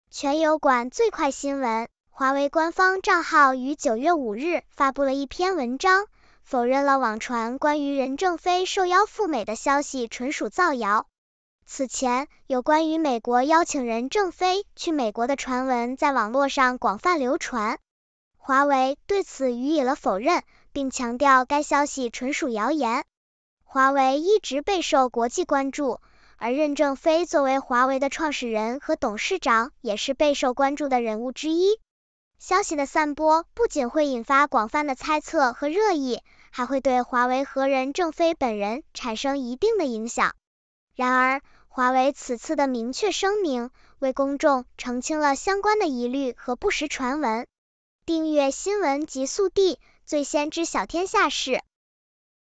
中国新闻09月05日20时：华为：网传“美国邀请任正非去美国”不实
华为官方账号于9月5日发布了一篇文章，否认了网传关于任正非受邀赴美的消息纯属造谣。此前，有关于“美国邀请任正非去美国”的传闻在网络上广泛流传。华为对此予以了否认，并强调该消息纯属谣言。华为一直备受国际关注，而任正非作为华为的创始人和董事长也是备受关注的人物之一。消息的散播不仅会引发广泛的猜测和热议，还会对华为和任正非本人产生一定的影响。然而，华为此次的明确声明，为公众澄清了相关的疑虑和不实传闻。